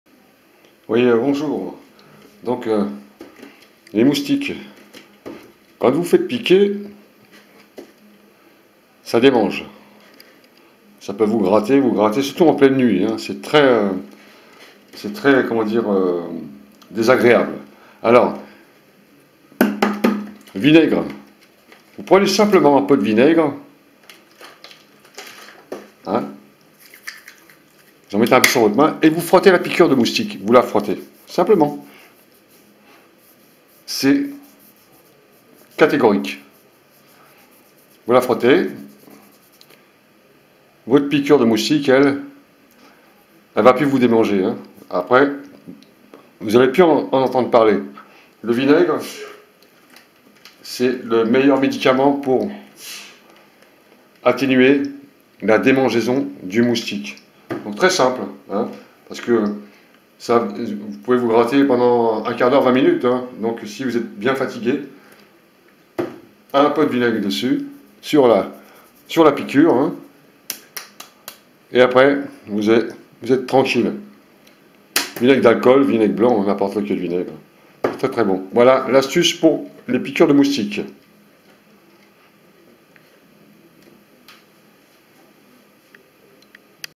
comment éliminer la démangeaison d'une piqûre de moustiques ,cela marche a 100 %
Le remède efficace pour en venir a bout de la piqûre de moustique un geste simple